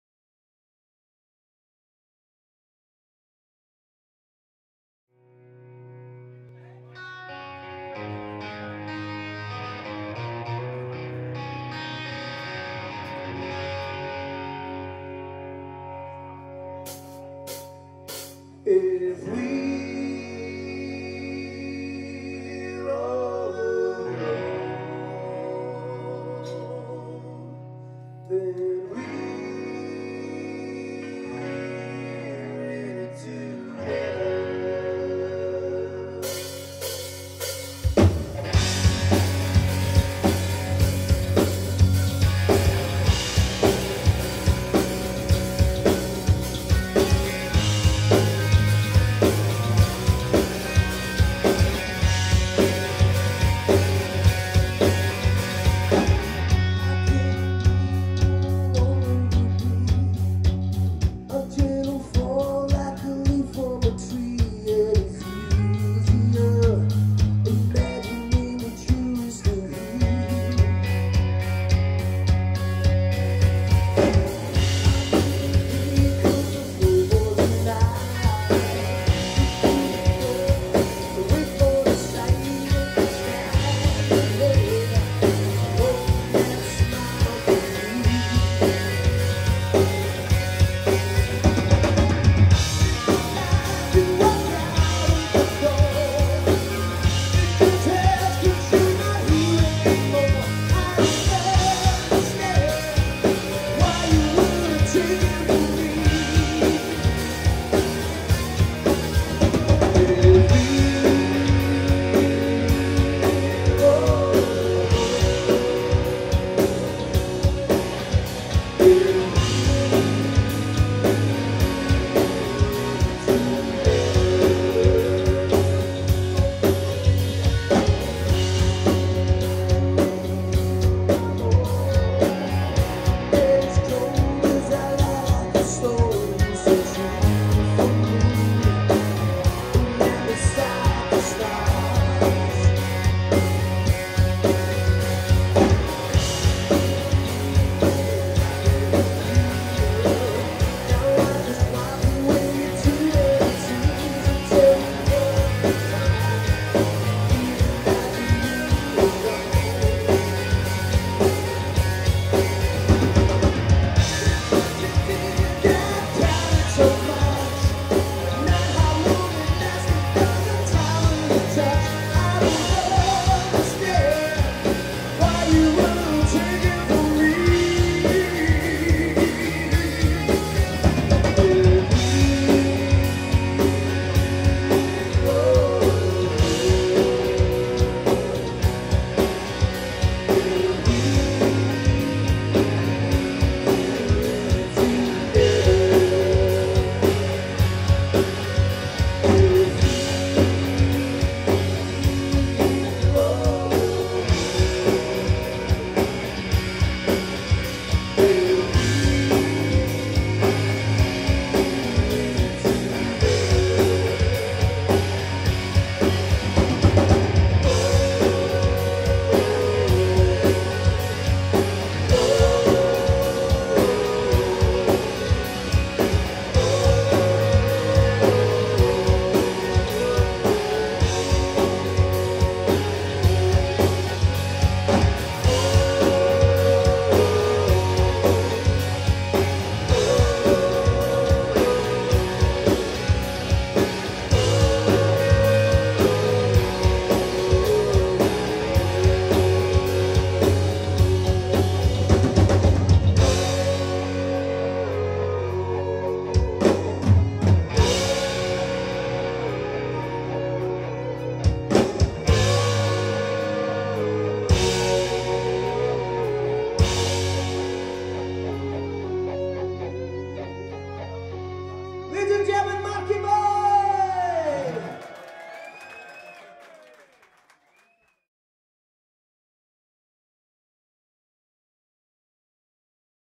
The King Lot with Marc Live @ Dreadnought, Bathgate, Scotland 12th February 2016
A very treasured recording on this night with The King Lot, Saint of Arcadia and Mason Hill, this one with Marc playing along side the King Lot. I did say a few weeks ago when he was in the Dreadnought I may surface a track or 2 (Marc) ;-)